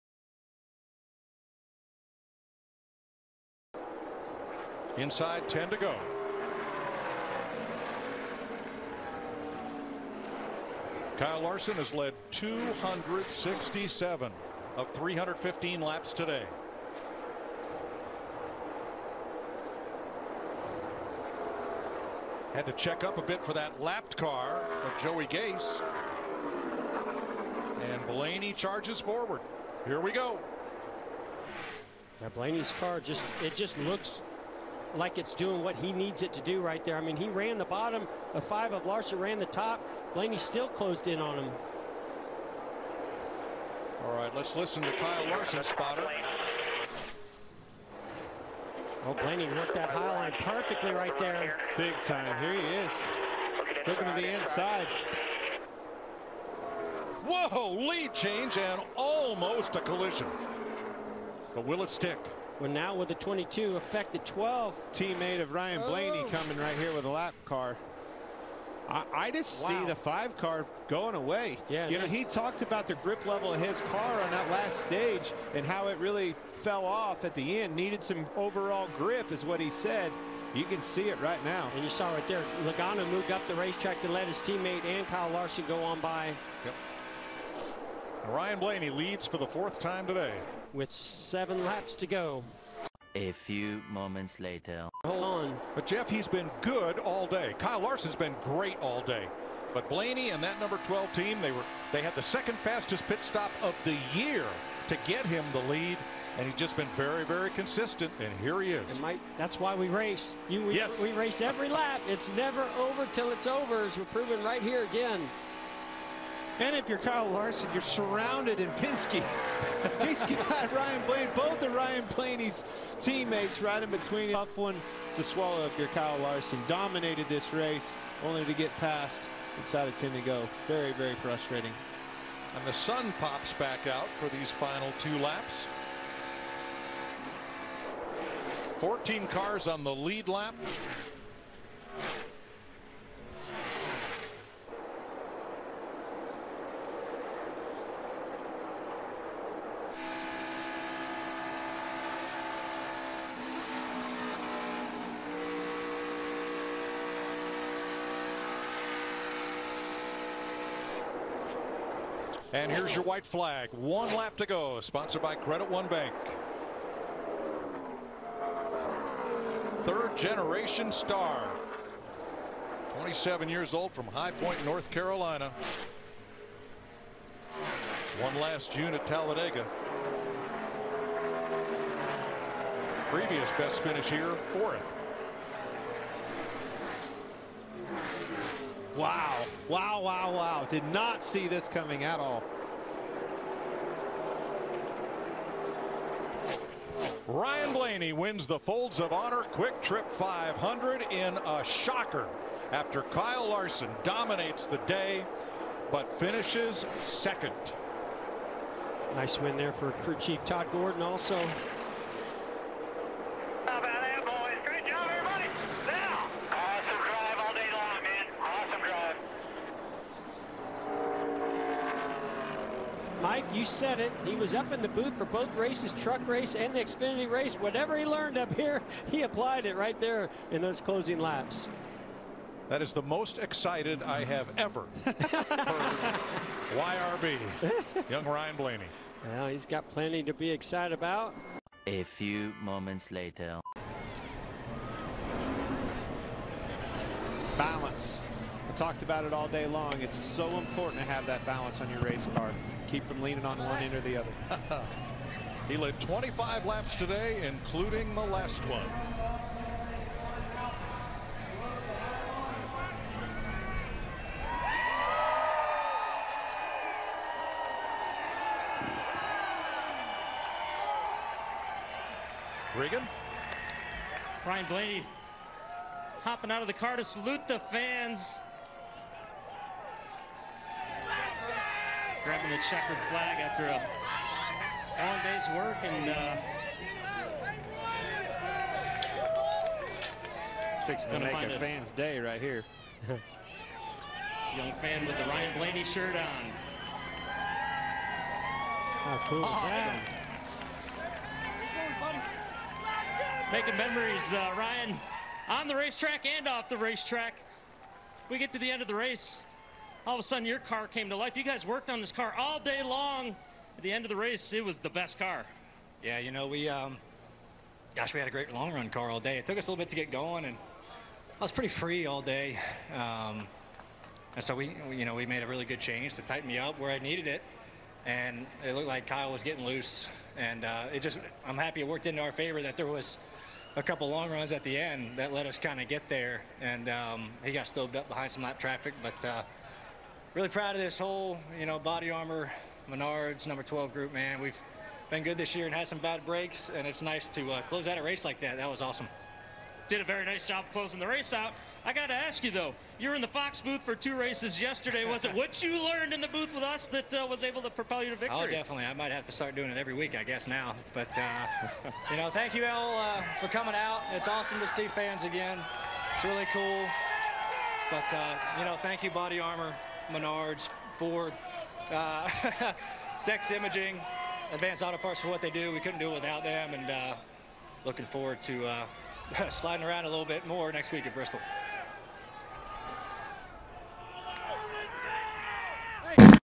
Blaney at Atlanta
Ryan Blaney wins at Atlanta

NASCAR

FOX SPORTS